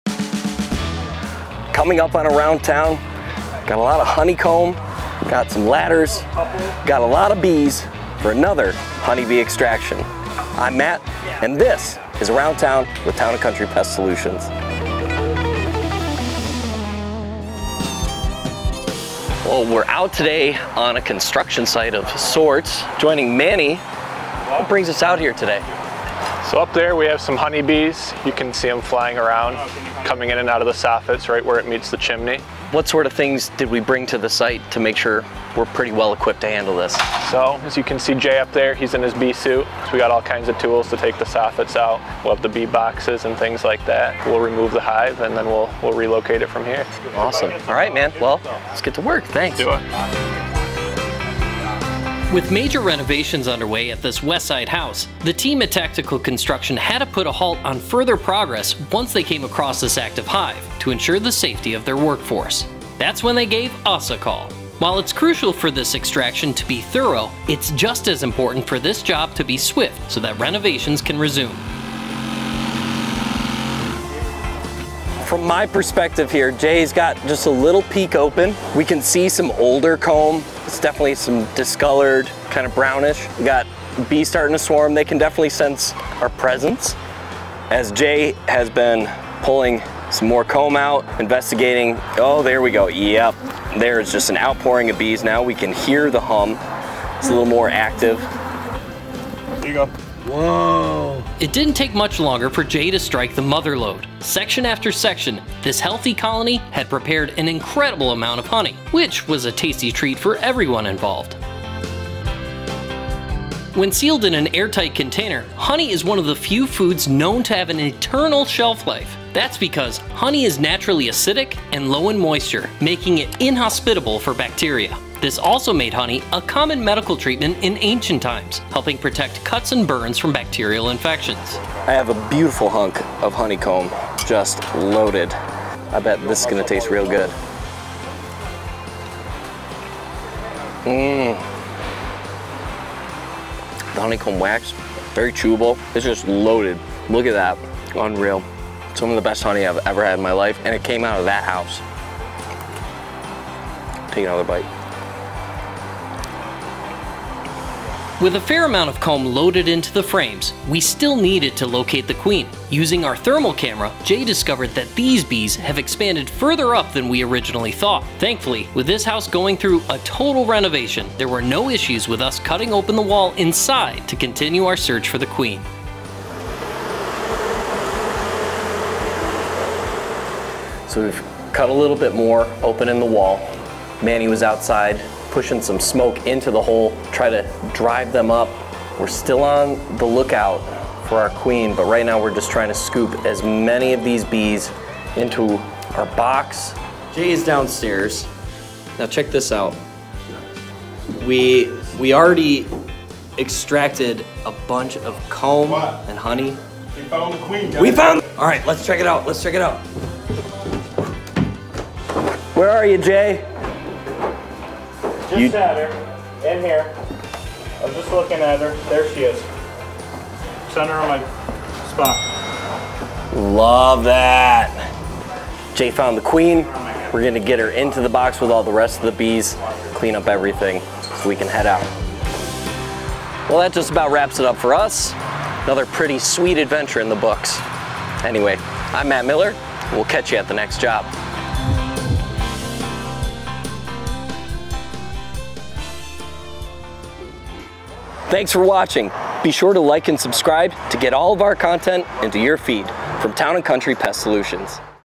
Honey Bee Rescue | Around Town
Getting the call from Tactical Construction, we gear up to extract a massive honey bee colony from a home renovation job site.

In this video, we show you what goes into extracting honey bees from a house under construction. We start by locating the hive, then safely and effectively remove the bees and their honey.

This video is for anyone who is interested in learning more about honey bees and how to safely remove them from a home. It is also a valuable resource for homeowners who are concerned about honey bees in their homes.

0:00  - Intro
0:17 - What We're Dealing With
0:51 - Why It's Important
1:14 - Opening the Hive
1:40 - Honey Tasting
2:40 - Finding The Queen
3:53 - See You At The Next Job